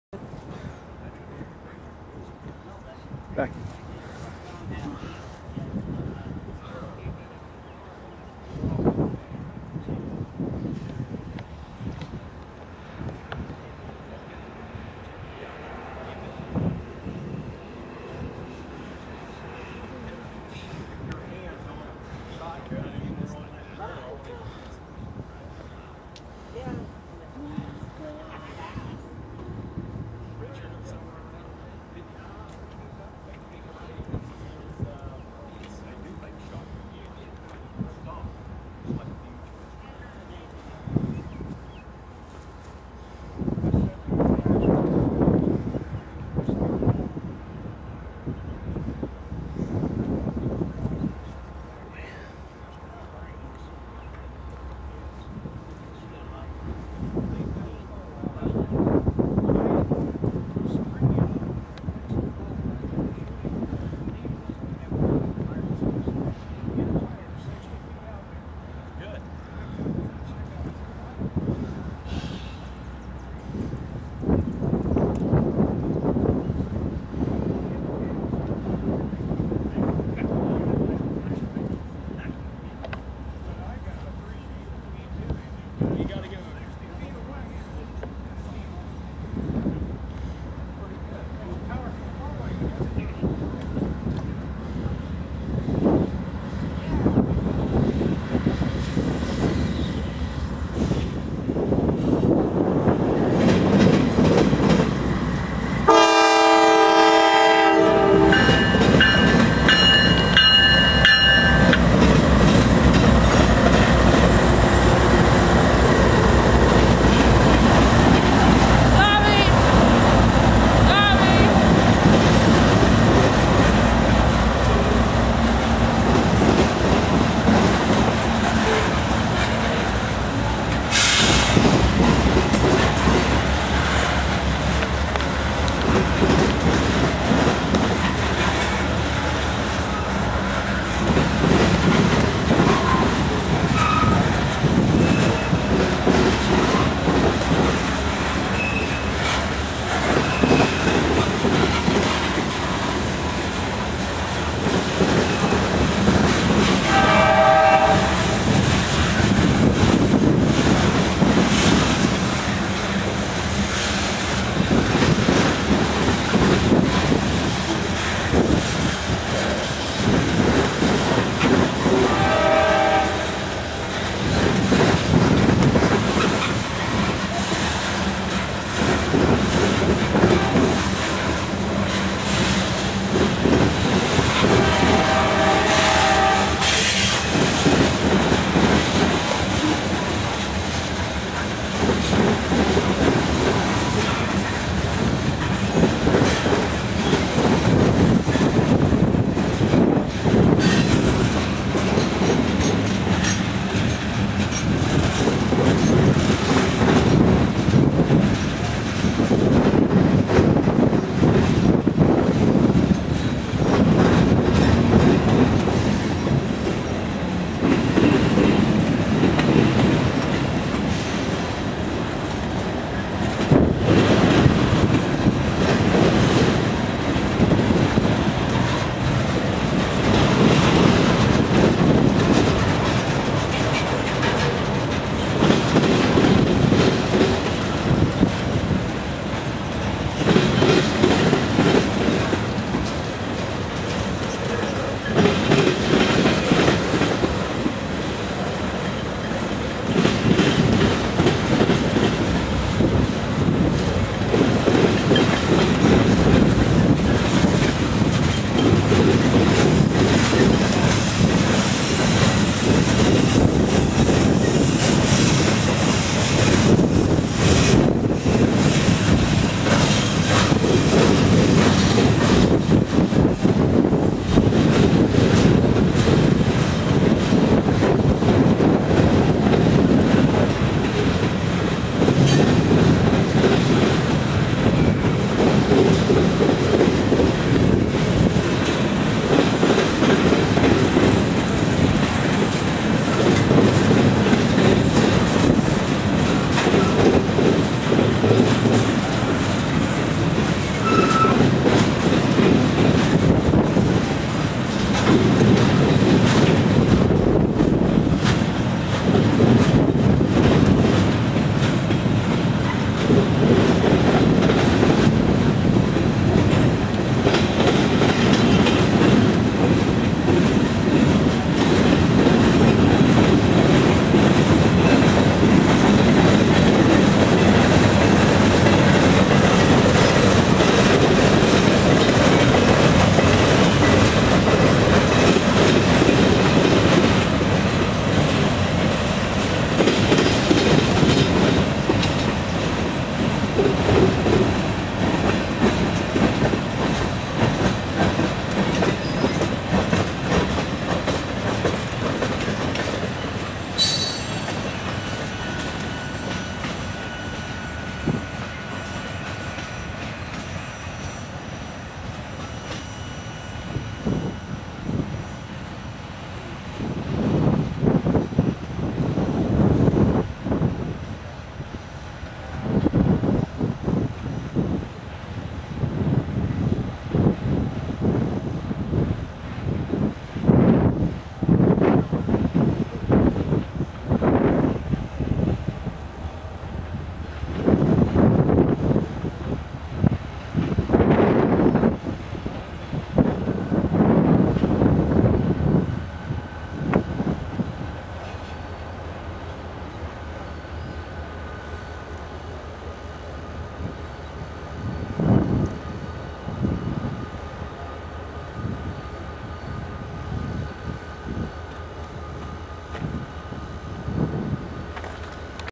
02 West Bound Mixed Cargo.wmv
A westbound mixed cargo train from the Wabash Yards, Decatur, IL.